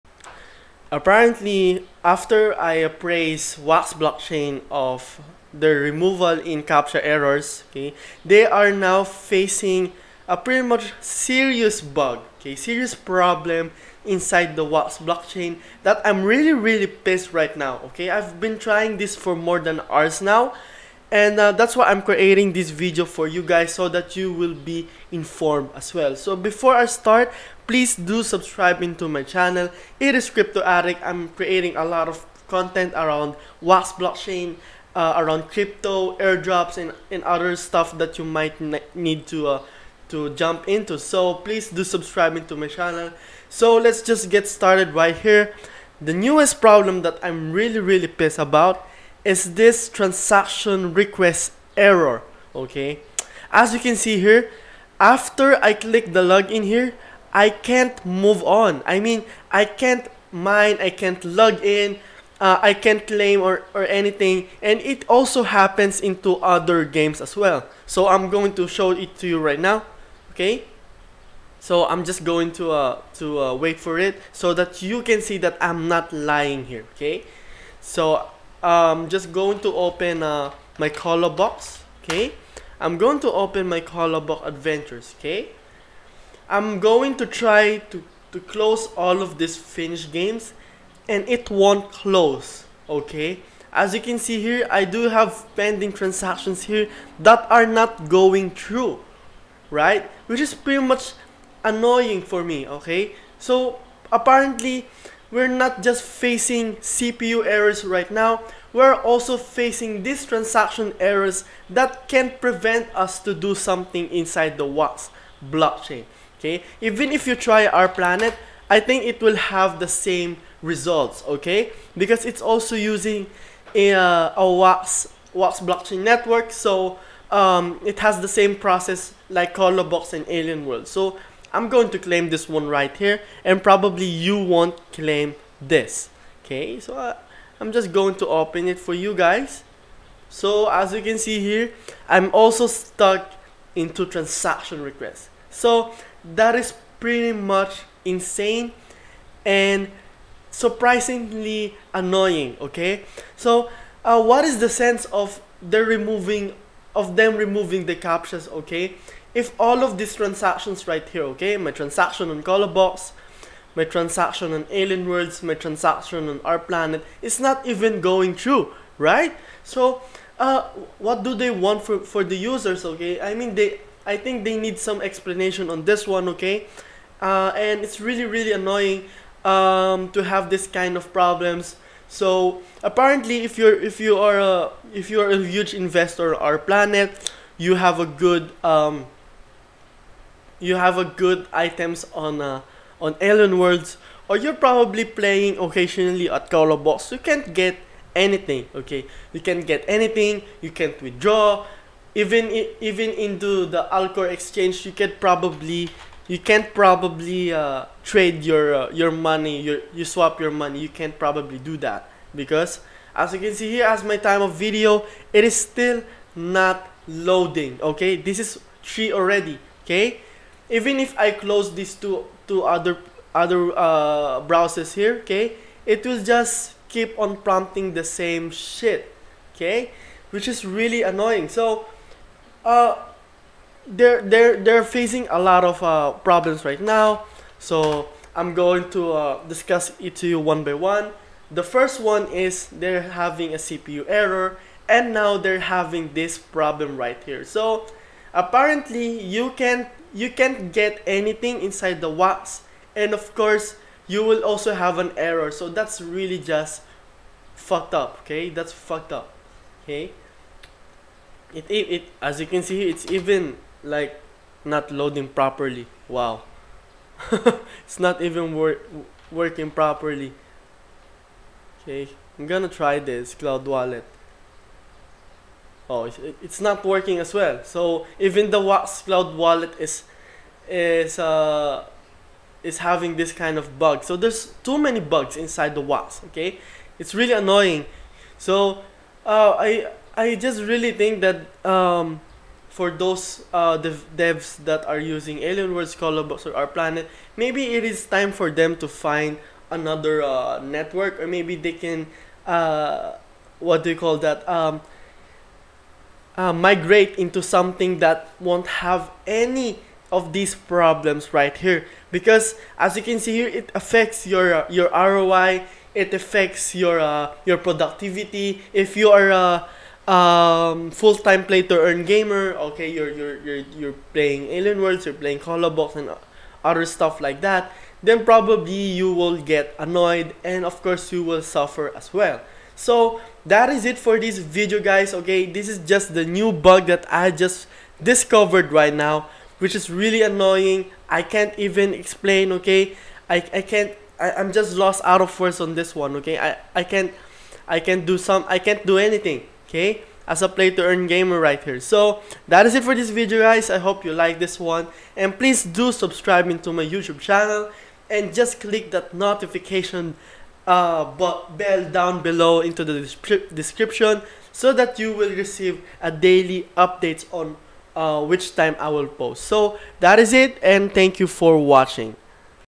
New Errors in WAX Blockchain PISSES ME OFF! | Transaction Stuck Error, CPU Error, Wallet Error
In today's video, I will be showing to you the new errors that makes me so PISSED inside WAX Blockchain. I've been waiting for hours for this and it is still not working for me.

Timestamps:
00:54 - Stuck in Transaction Request (NEW ERROR ON WAX)
02:35 - Captcha Removal is USELESS?
04:00 - Wax Cloud Wallet Logging In Loop ERROR
04:30 - Never ending CPU Errors
04:57 - Open Letter to Alien Worlds R-Planet and Kolobok Adventures Developers